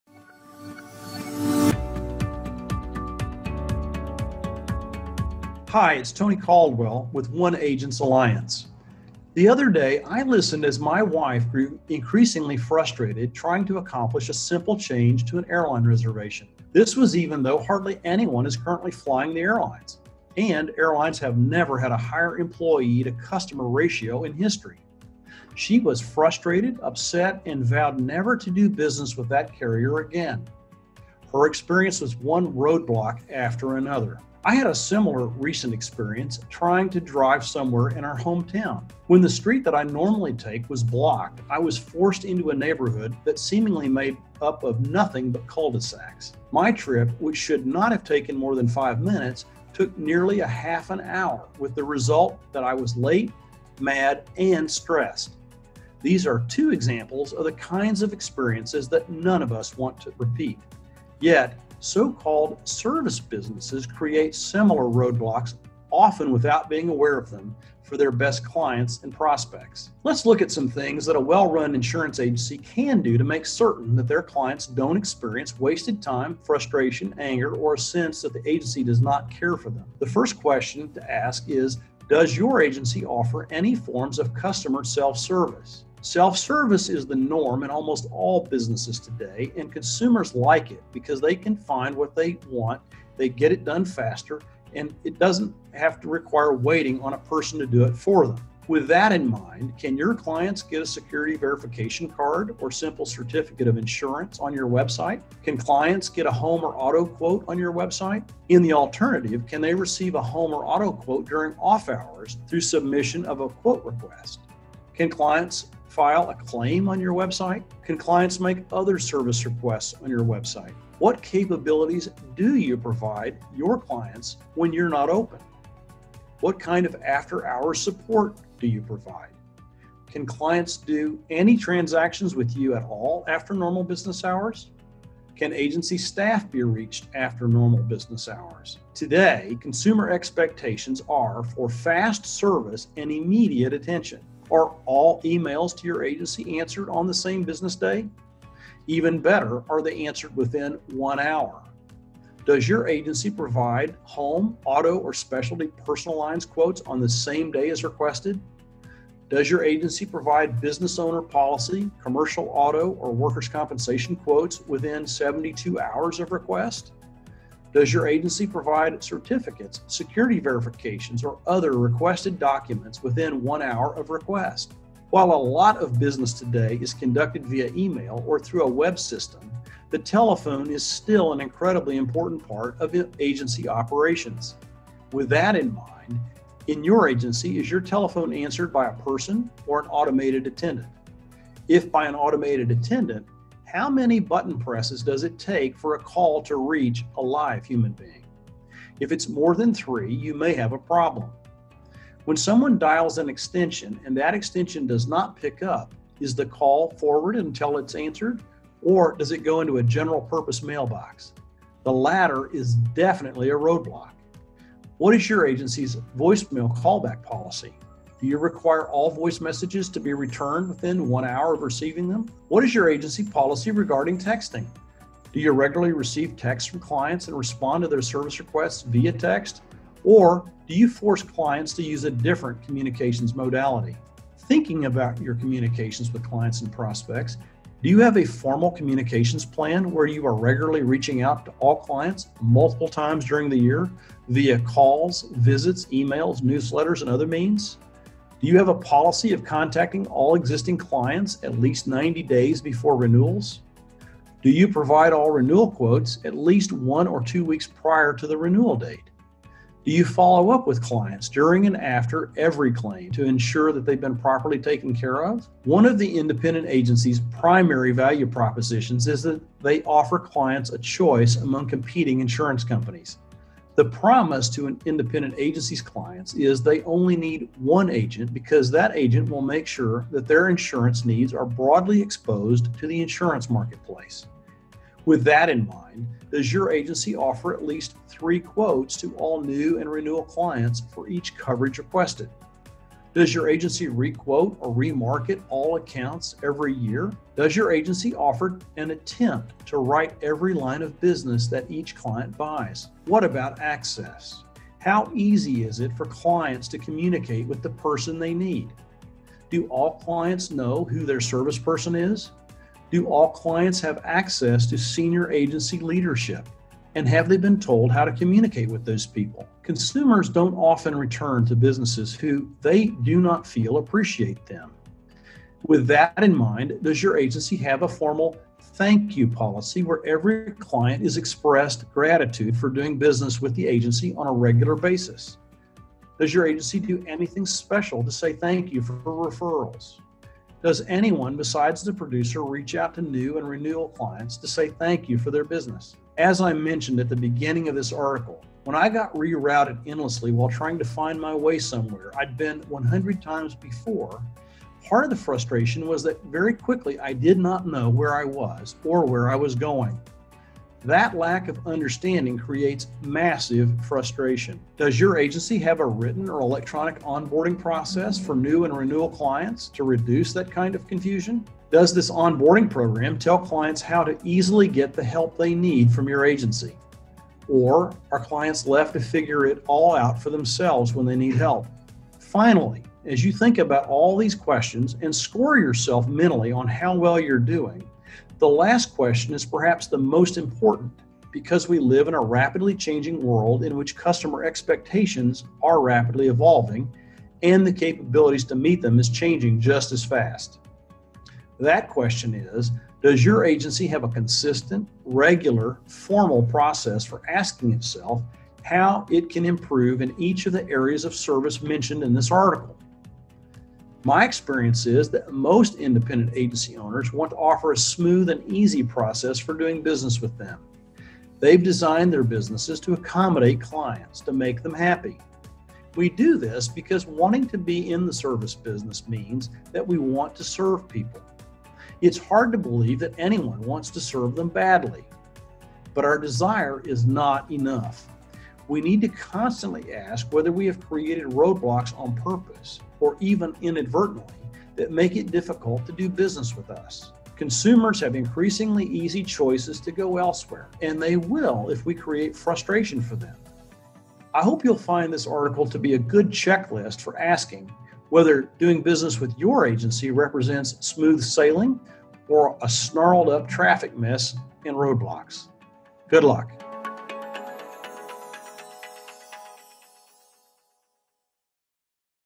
Growth Agent Insight - Roadblocks to Success
This week on Growth Agent Insight, CEO Tony Caldwell talks about roadblocks.  Not just roadblocks in life but roadblocks to your business and to your dreams and goals, and what you can do to avoid these roadblocks.